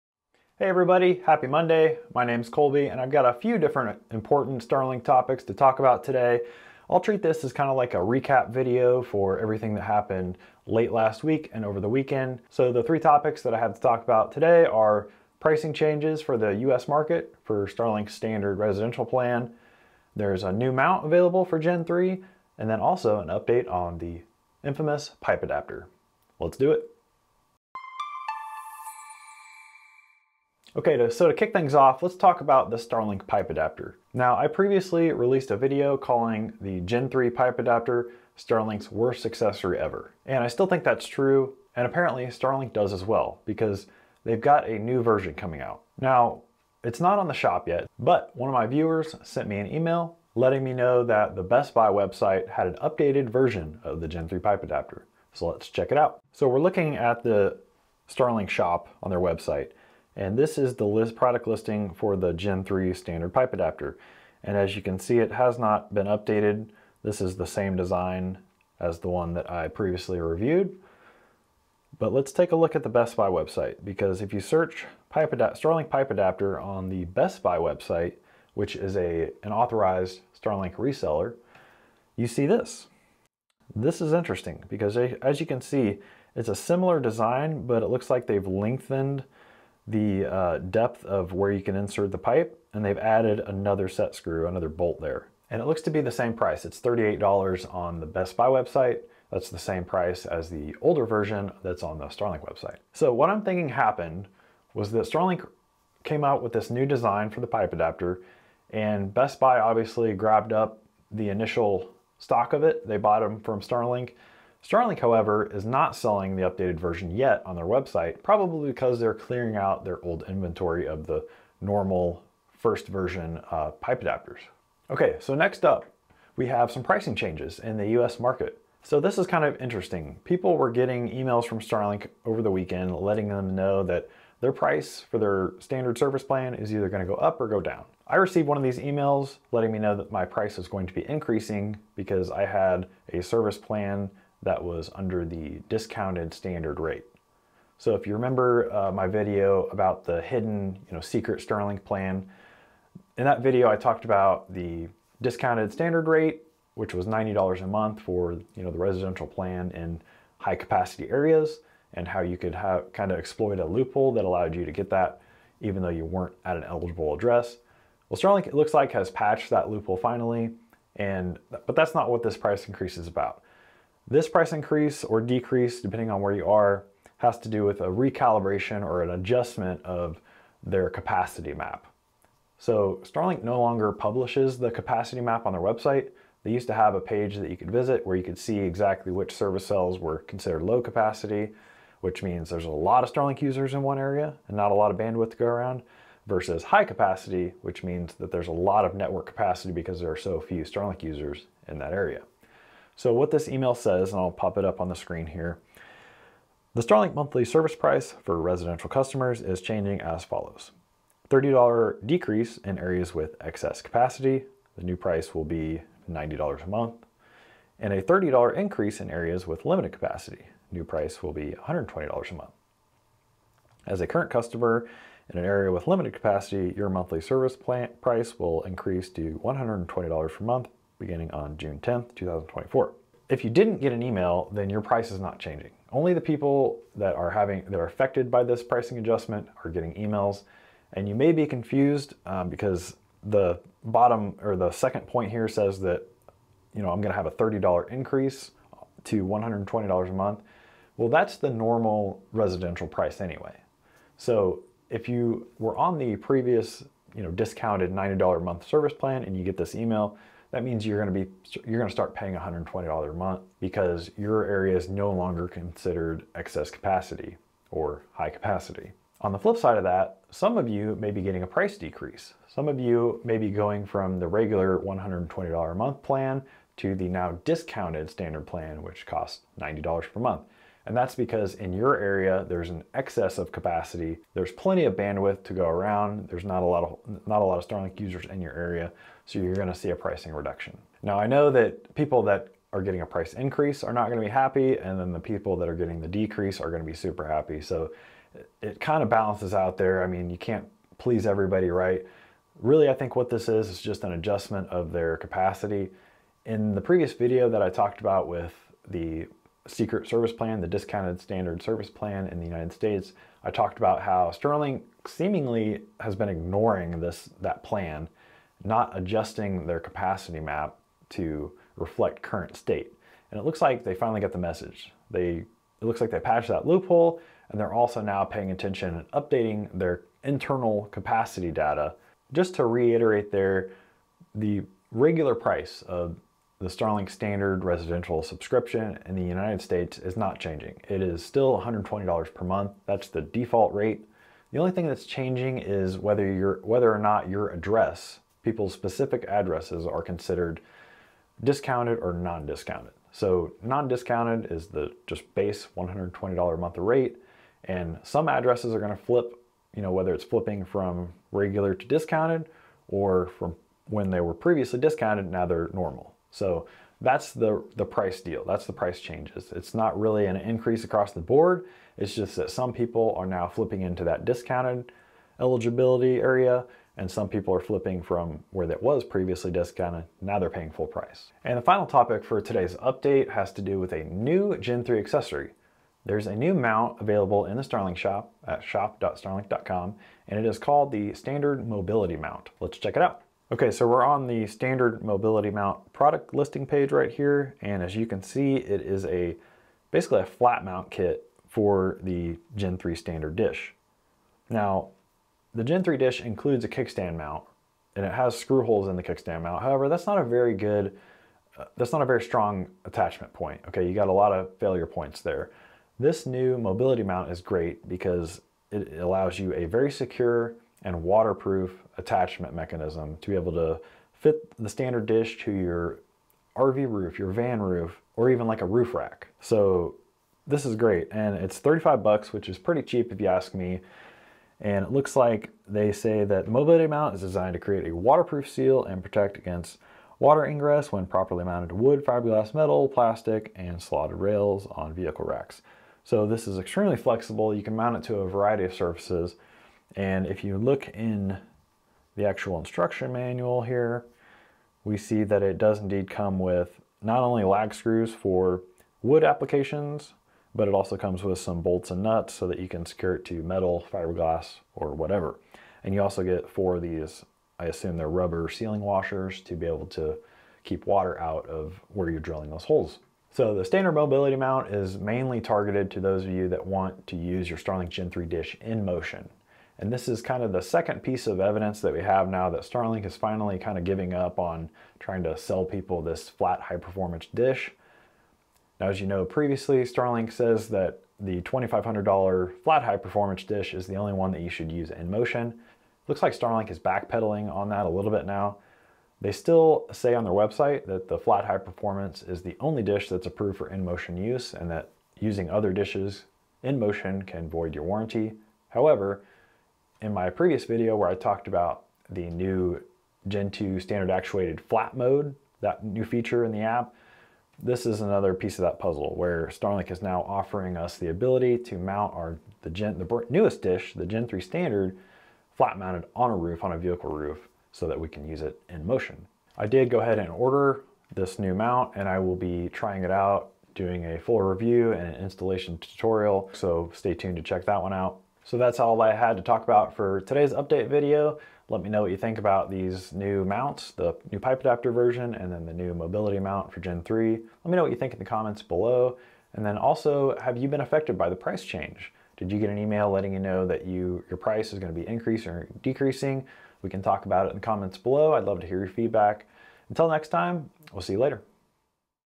Starlink price change + new mounts!
Starlink is adjusting their high capacity and low capacity areas. I tell you what it means for your monthly service price, plus I check out the updated Gen 3 Pipe Adapter. Finally, I cover the brand new Mobility Mount for Gen 3.

I use Private Internet Access (PIA) for my VPN service.

**Gear that I use with Starlink**
eero Pro 6E Mesh System
TP-Link 8 port switch
Powerline adapters